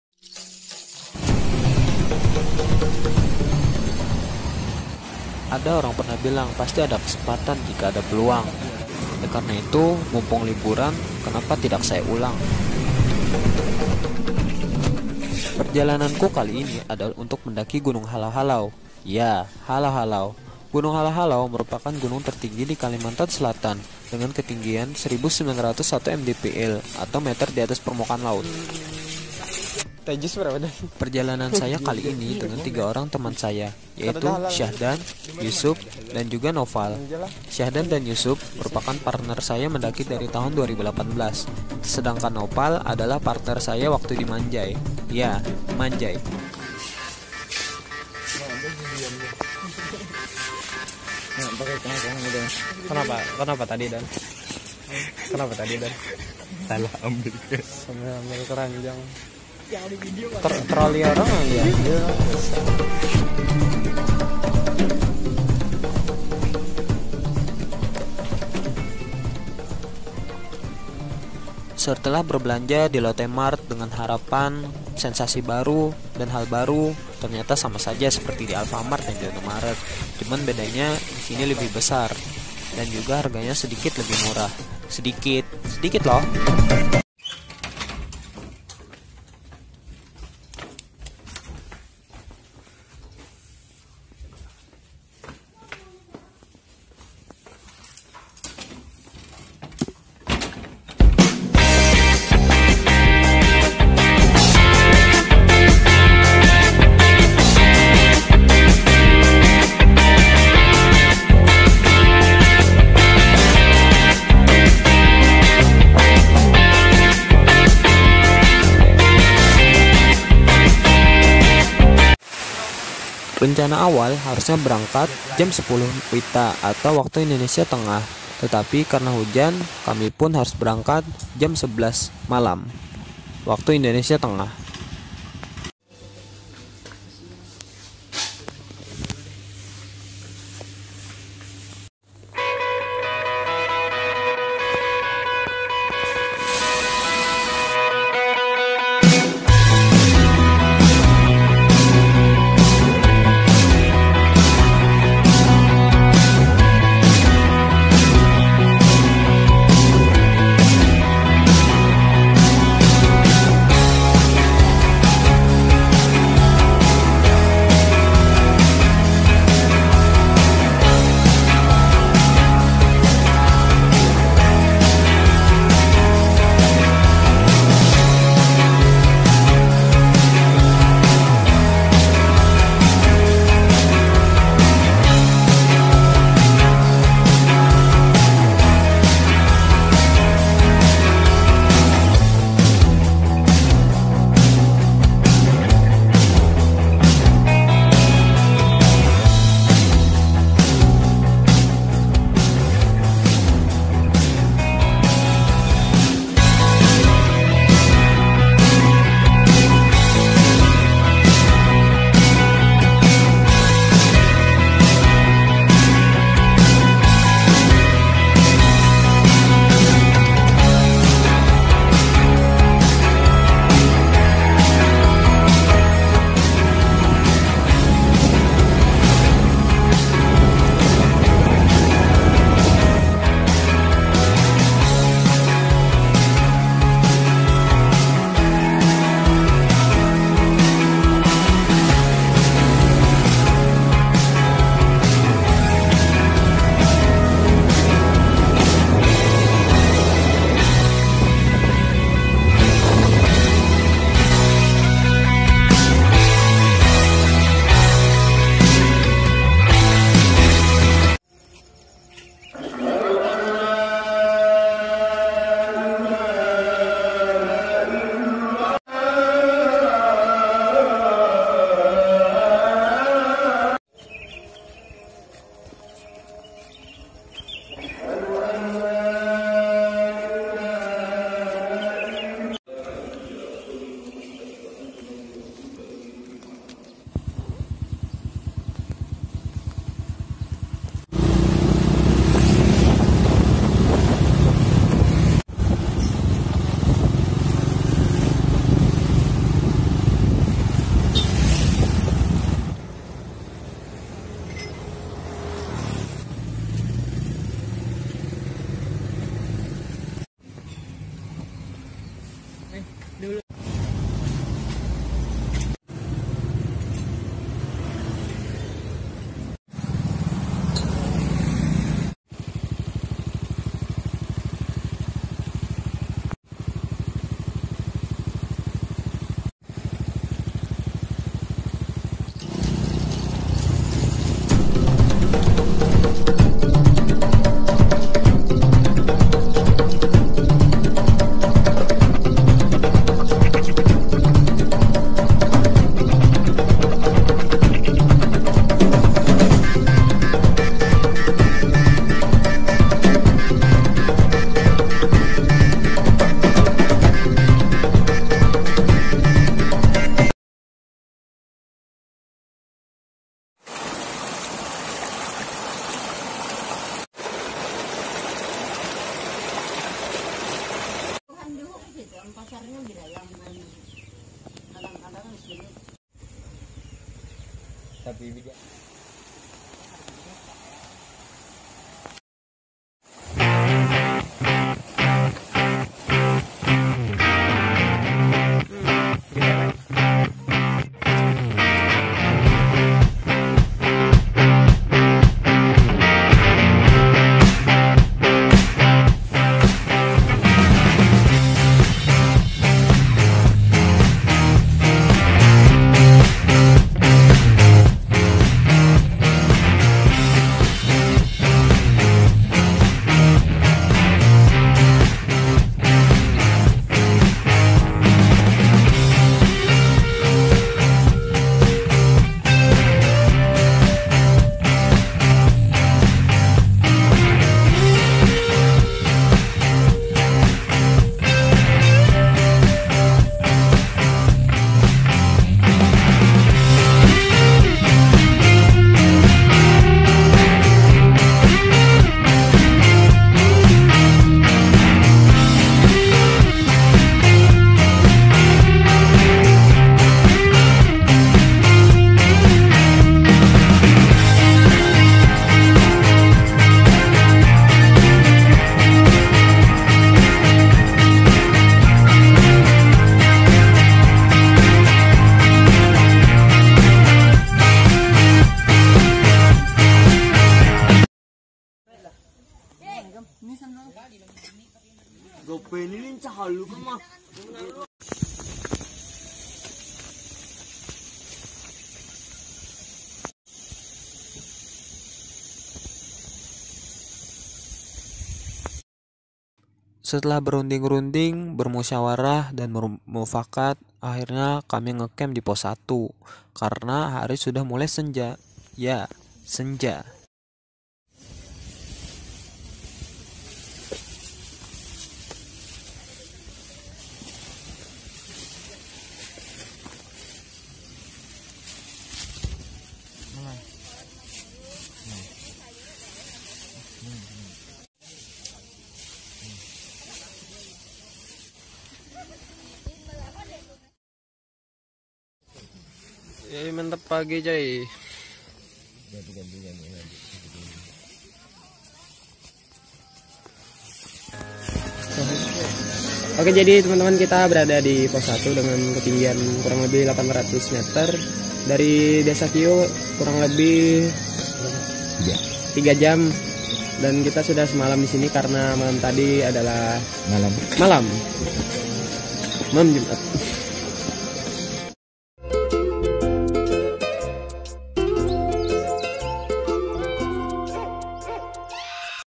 MIUVID 9 - MT. HALAU HALAU 1901 Mdpl via Desa Kiyu (Part.1)
Gunung halau halau merupakan gunung tertinggi dikalimantan selatan, dengan ketinggian 1901 meter diatas permukaan laut. Gunung halau halau terletak di Batang alai timur, Kabupaten Hulu Sungai Tengah, Kalimantan Selatan. Kali ini aku mendaki via Desa kiyu, perjalanan dari Kota banjarmasin ke Desa Kiyu ±4 Jam 30 Menit-an
Instagram Syahdan
Instagram Naufal
Instagram Yusuf
Sampai Jumpa di Next Part✨
Jangan lupa bersyukur, mari adventure.